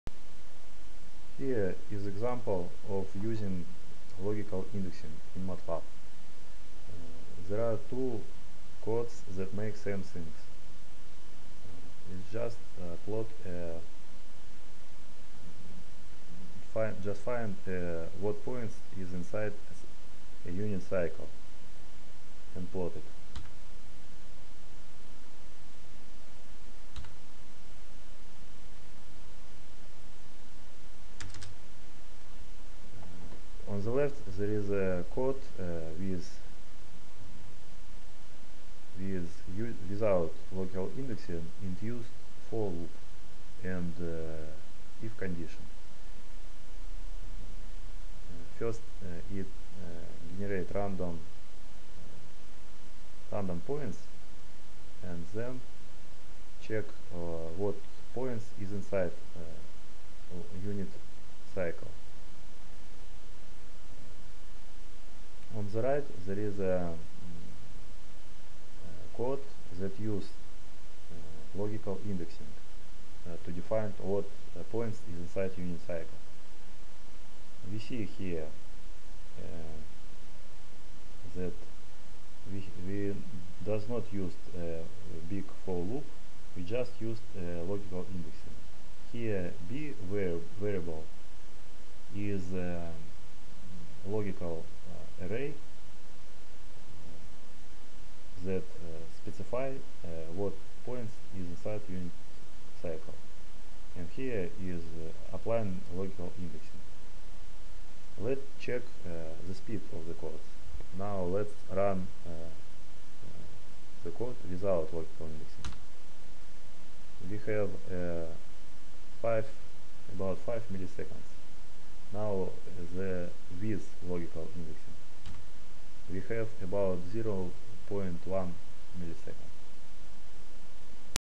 How to think vector in Matlab, part 2, logical indexing
There are two codes: without logical indexing and with logical indexing (no for loop). Both codes make same thing: check what points inside unit circle. Processing time compared.